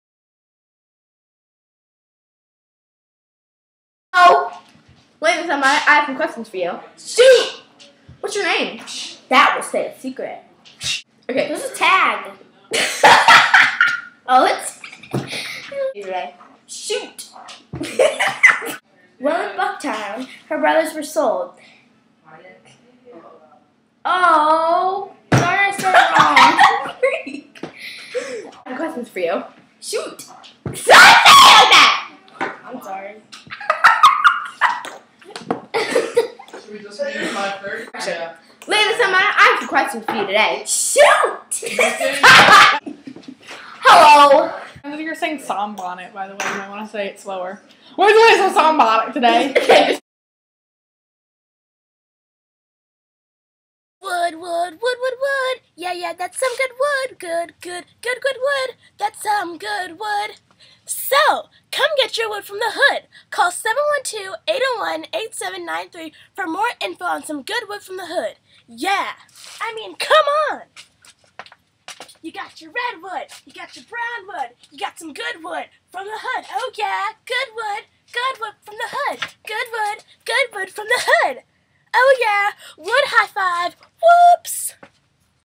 Bloopers
haha these are the bloopers to a harriet tubman project we had to do for school. Good Times Right Here.